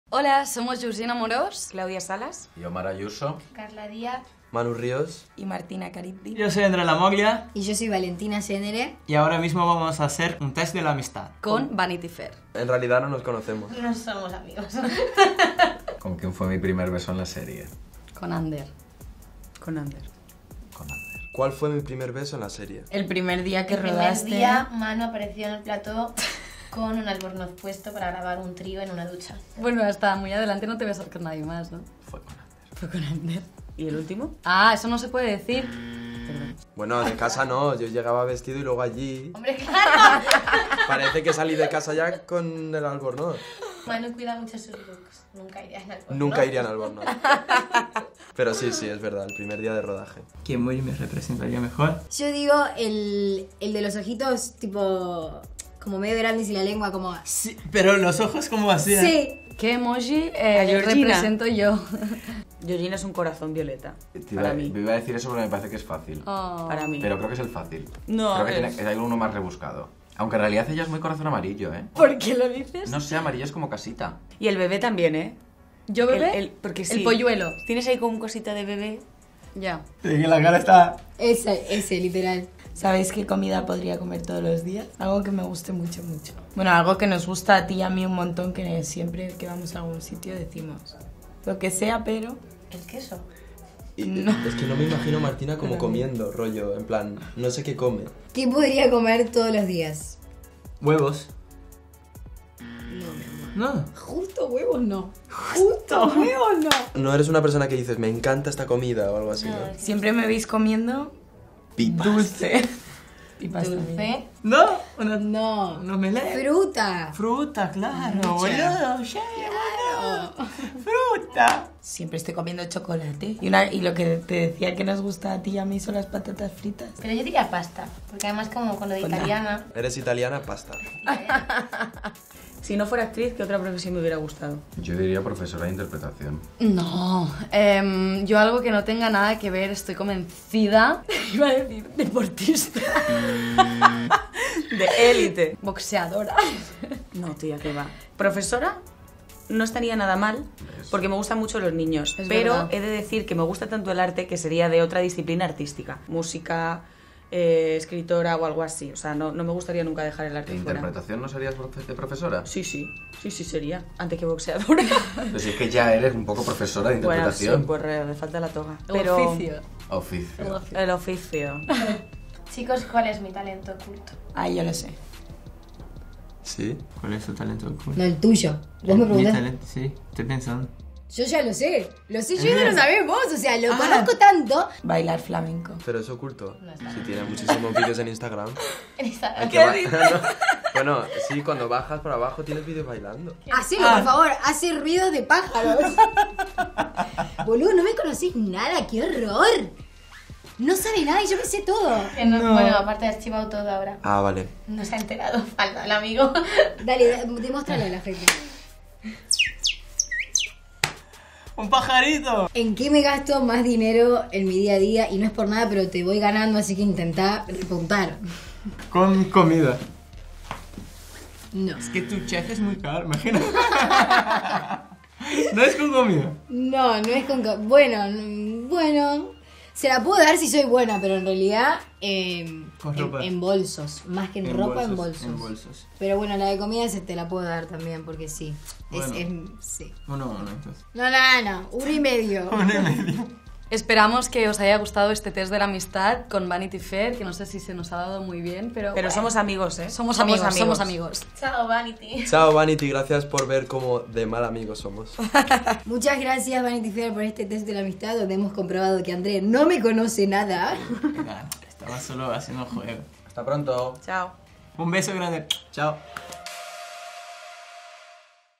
Valentina Zenere, Manu Ríos e il resto del cast di 'Elite' fanno il test dell'amicizia
Manu Ríos, Valentina Zenere, Georgina Amorós, Claudia Salas, Omar Ayuso, Carla Díaz e André Lamoglia mettono alla prova chi conosce meglio i loro compagni di cast nella serie "Elite", disponibile su Netflix nella sua quinta stagione.